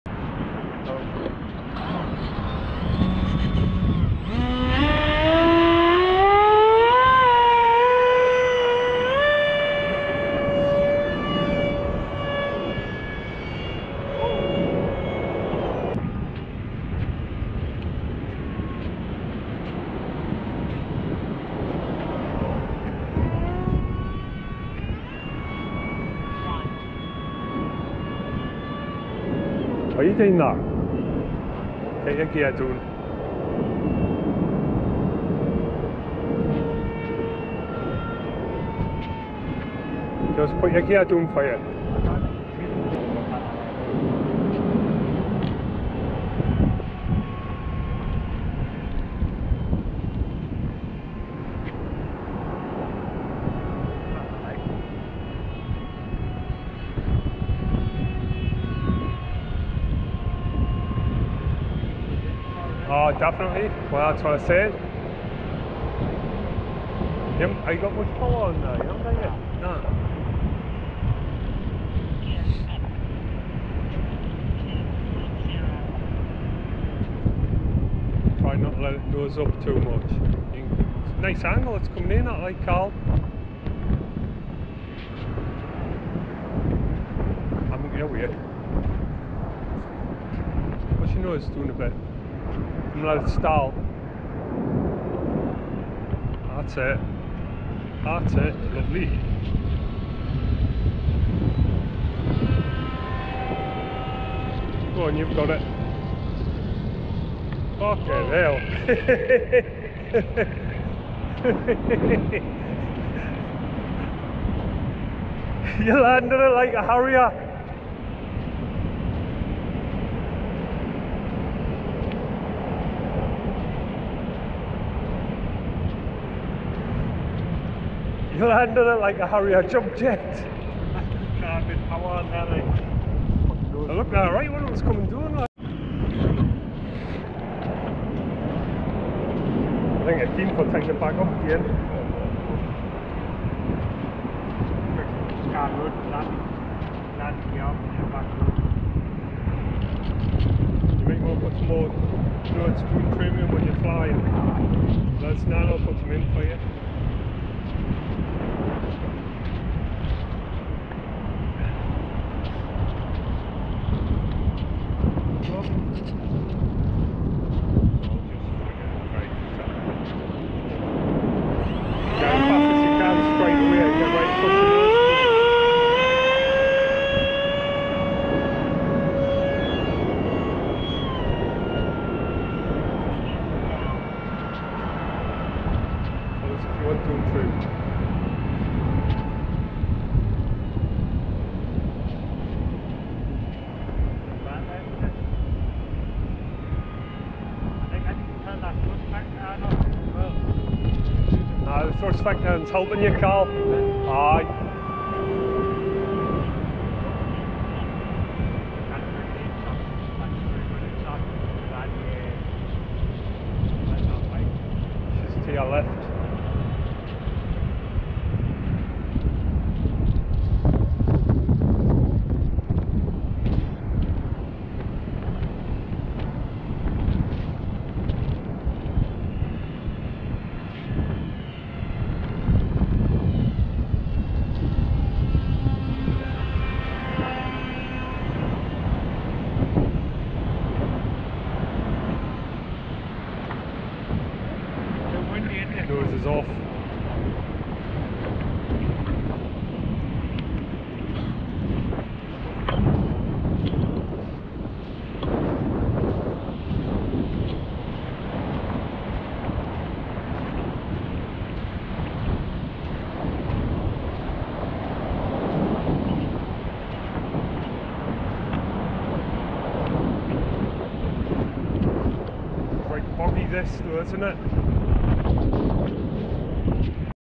freewing F35 kite
My brother Kal flying 2nd flight with his F35 , I told him it was too windy .